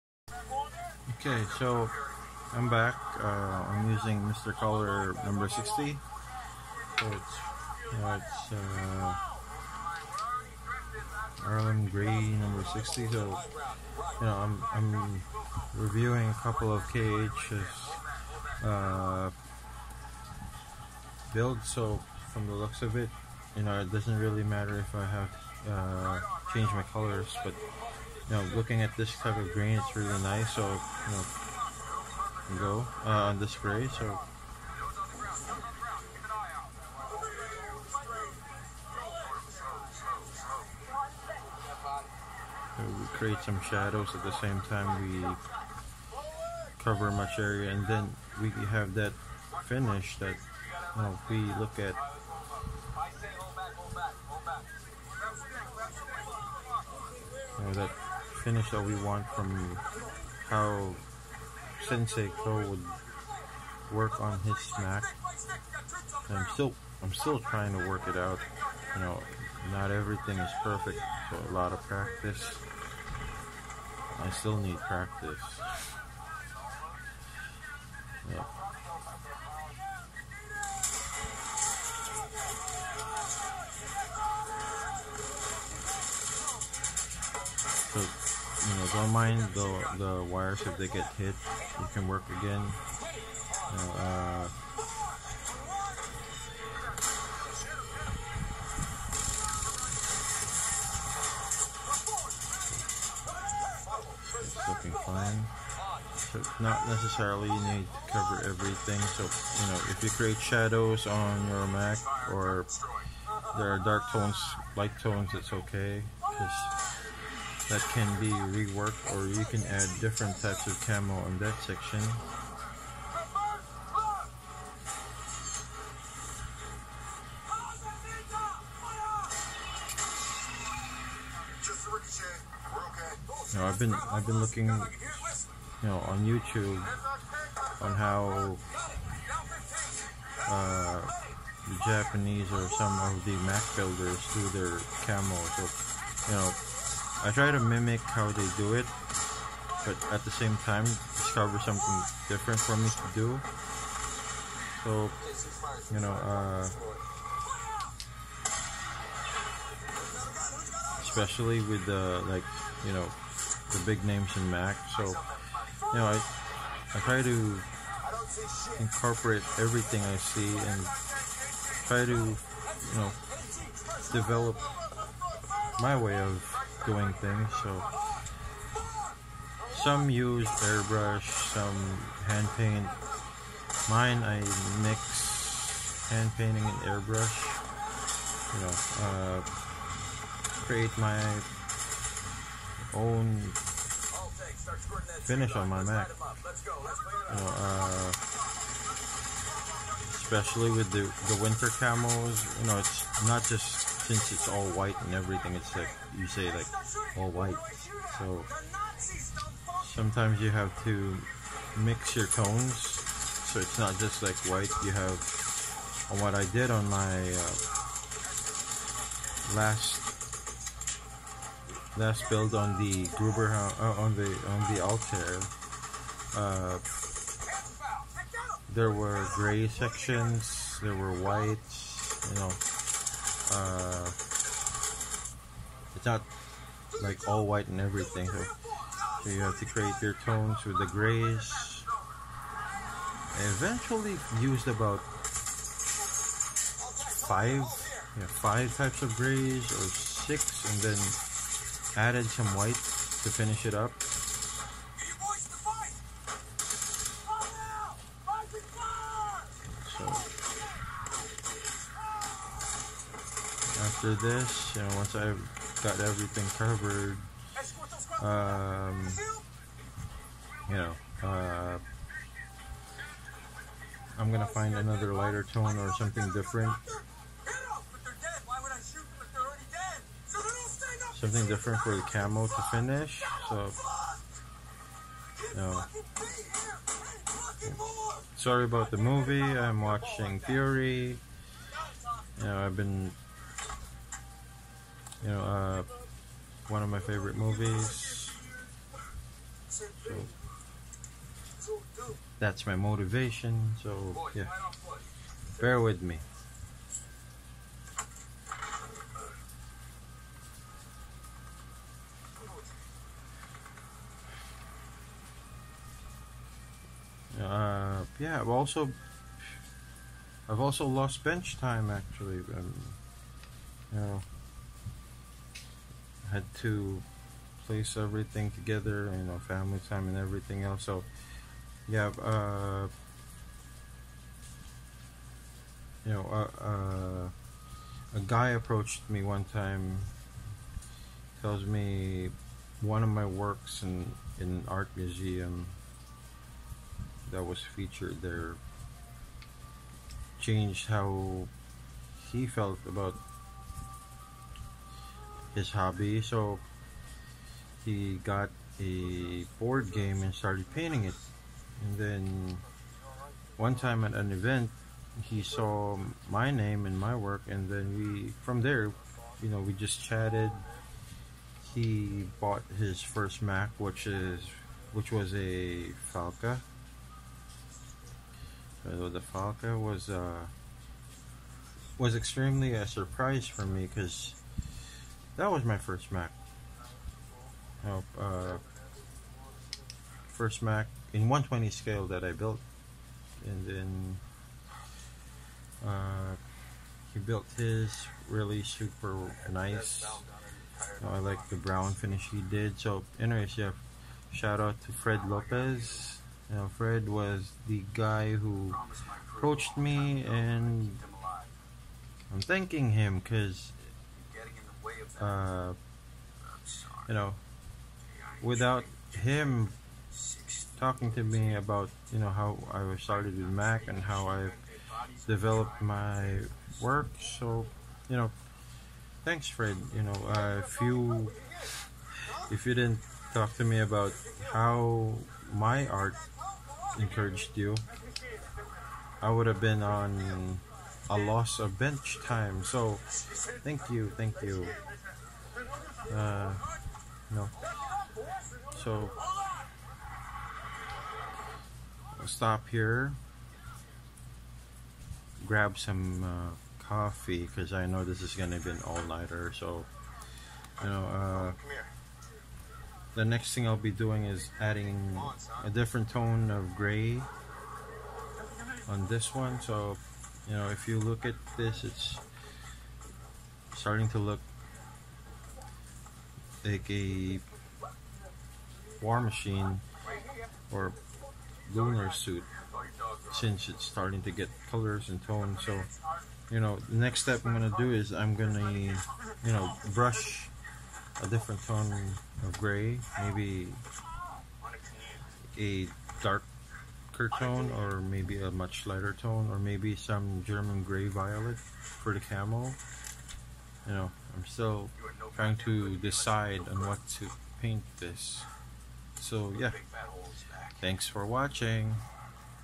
Ma.K MK-44 Heavy Luna WhiteKnight
After applying black primer by Zurc Scale Model Paint, its time to add the base tone. I used Mr Hobby IJN Green as base and also a regular brush to create the “Massey”Krieger look to the unit. In which a certain motion of the brush creates shadows and tones that “Harmonize” the armor effects.